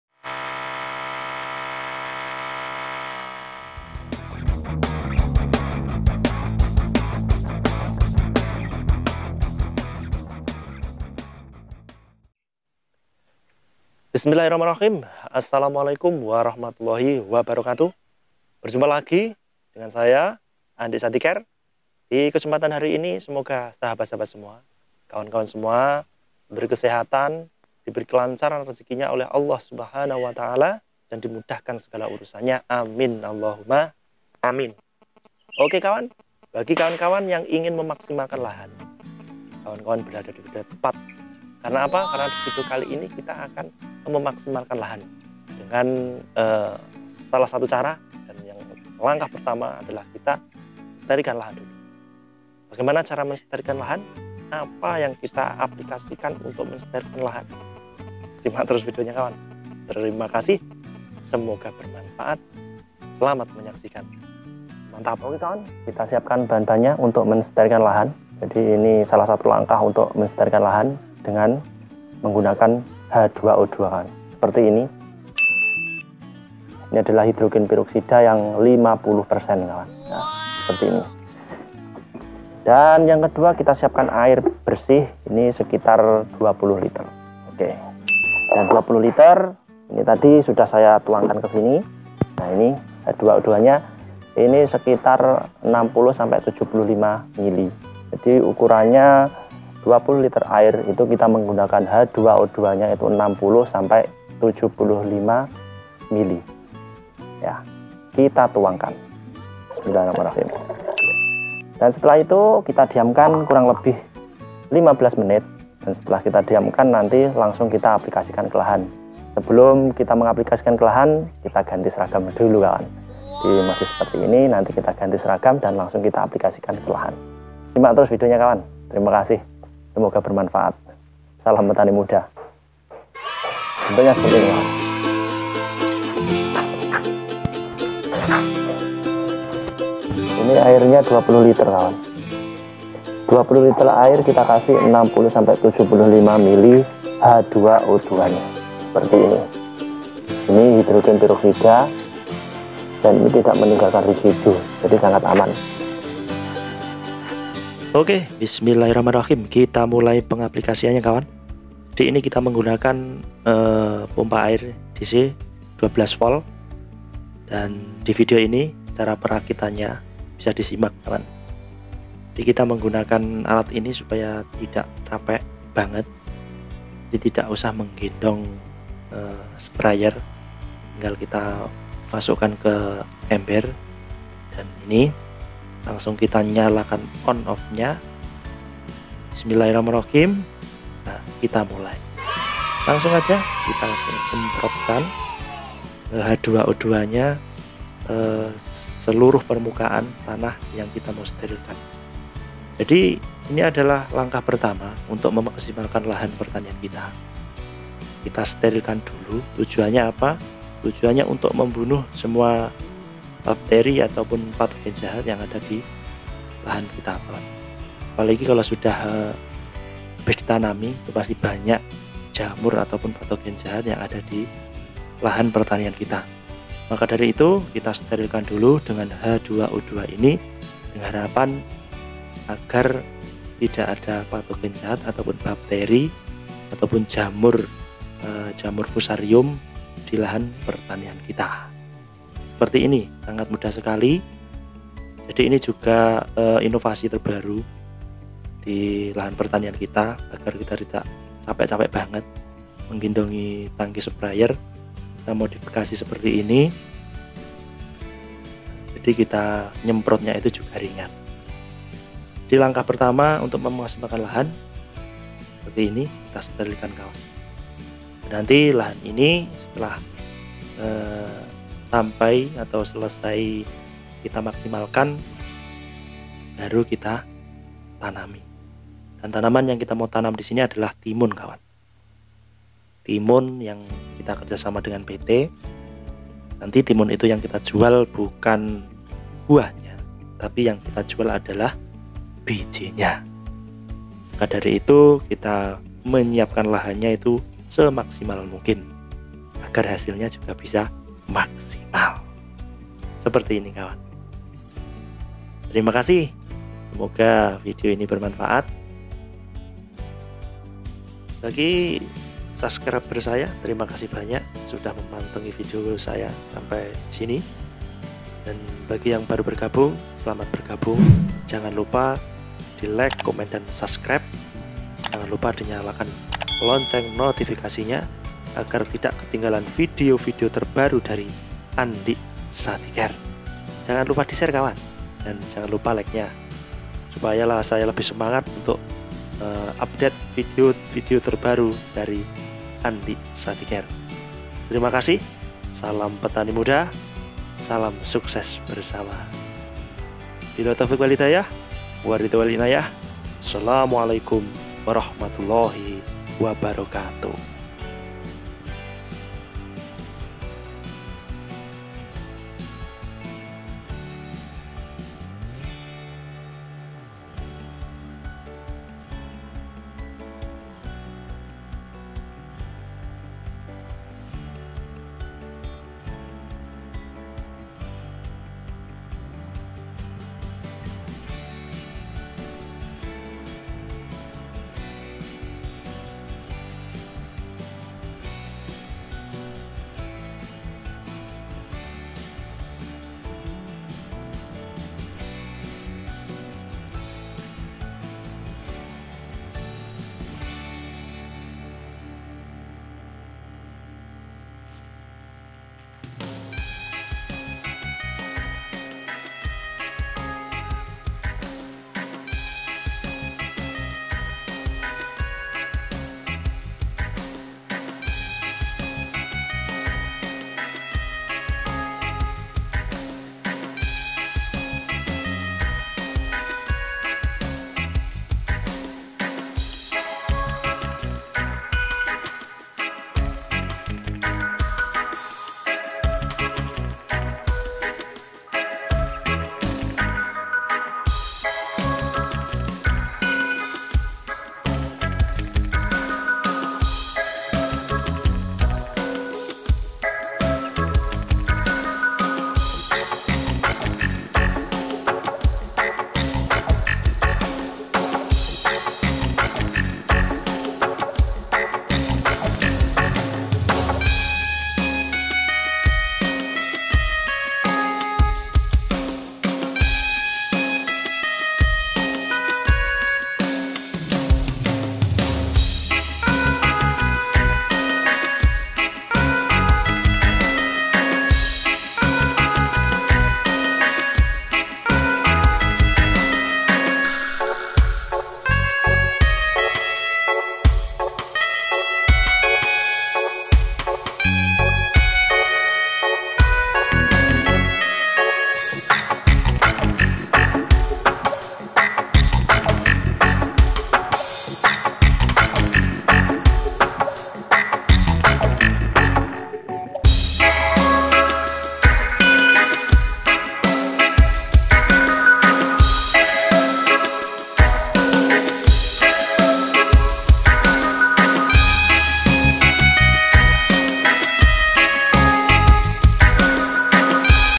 CARA MENYUBURKAN TANAH LAHAN PERTANIAN AGAR MAKSIMAL part 1
Di vidio kali ini kita akan memaksimalkan lahan pertanian dengan cara menstrilkan  lahan pertanian terlebih dahulu dengan H202 .Tujuanaya agar jamur fusarium,dan bakteri/patogen jahat bisa mati dan tidak tumbuh di lahan pertanian kita.setelah kita seterilkan semoga tanah menjadi gembur dan subur dan bebas dari penyakit.